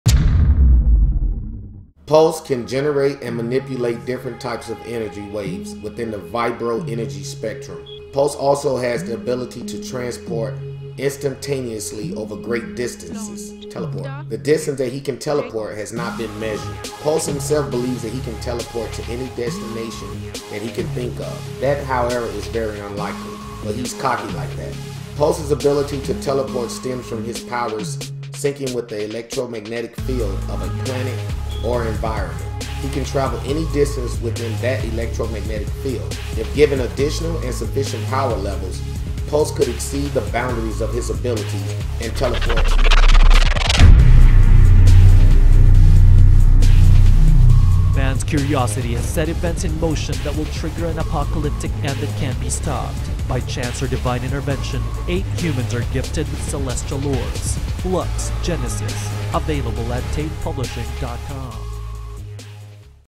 Pulse Character Profiling - FLUX: Genesis
FLUX: Genesis
W. L. White III

Federal Laws Under the Xtreme! (FLUX)

Man should have kept the earth hidden; now that they know, the extinction will begin.

We are not alone. Our planet is teeming with bio fuel for a species that outnumbers us a million to one, and we are not prepared. The enemy is innumerable, stronger, smarter, larger, and hungry; they could consume the very planet itself. Man's curiosity has set events in motion that will trigger an apocalyptic end that can't be stopped. By chance or divine intervention, eight humans are gifted with celestial orbs. Their story begins in the pages of FLUX: Genesis; Intech, Pulse, Friction, Phase, Flame, Hydro, Shift and Atom. Will their new power be enough to stop the onslaught of this truly alien foe?

@flux_chaos on twitter